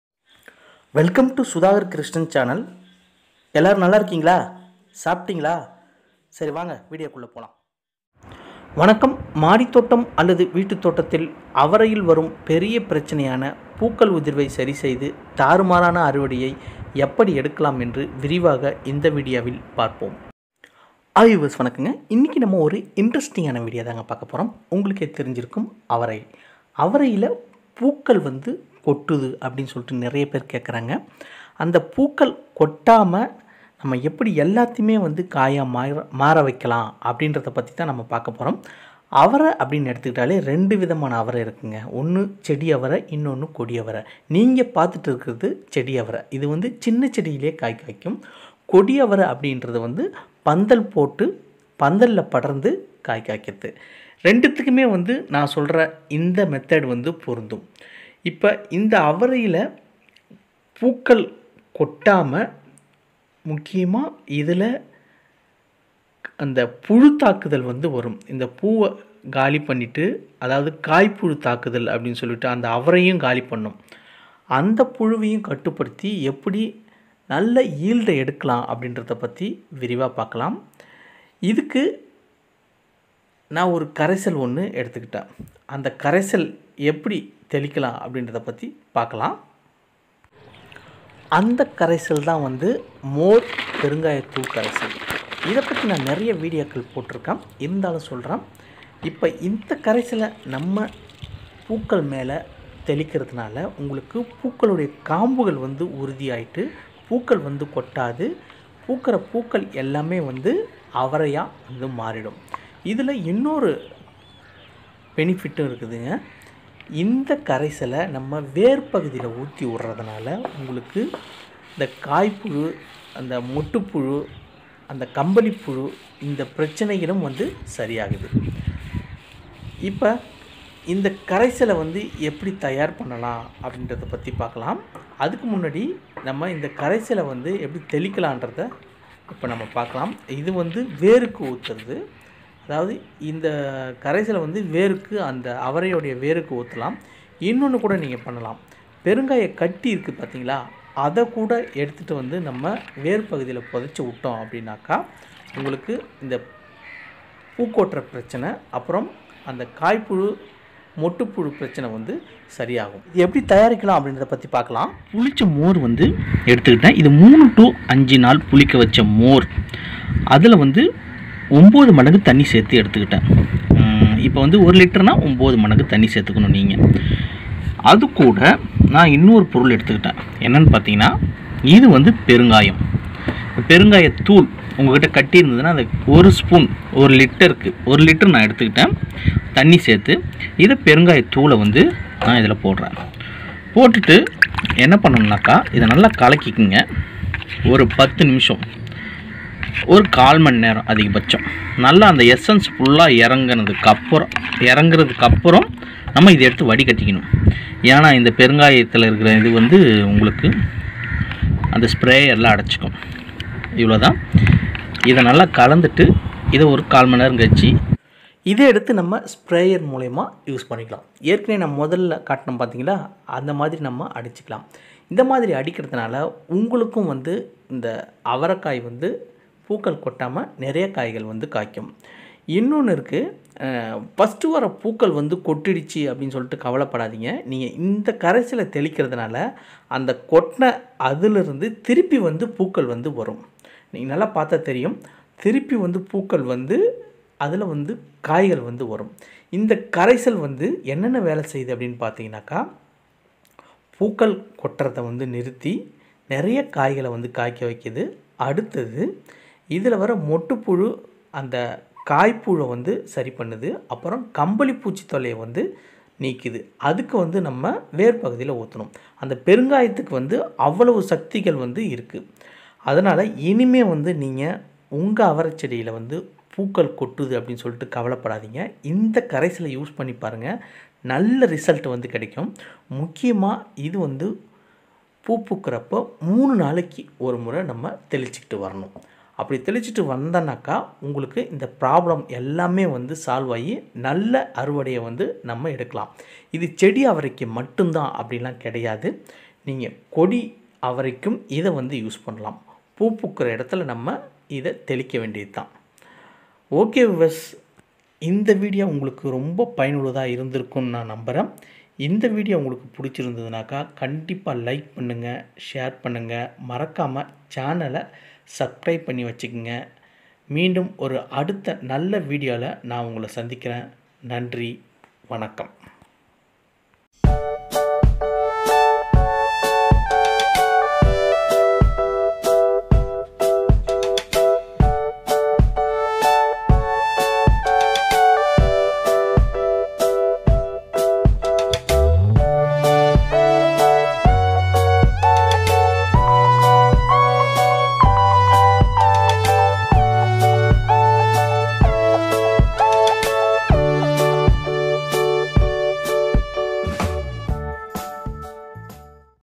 How to Easily fix the Problem of "Falling Flowers" on Lablab beans & get a High Harvest?
The biggest challenge is the shedding of flowers on the lablab beans.  This video gives you an easy way to control flowers falling off  and get more Vegetables.